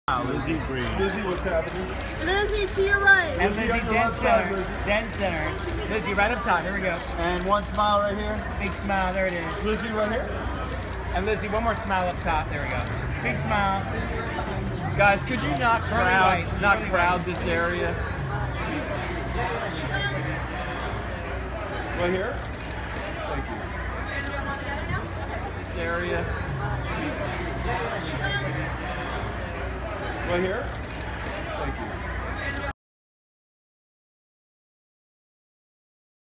Lizzy Greene arriving at the Kids Choice Awards in Inglewood CA
Lizzy Greene arriving at the Kids Choice Awards in Inglewood CA. To see all the attendees and photos from this event go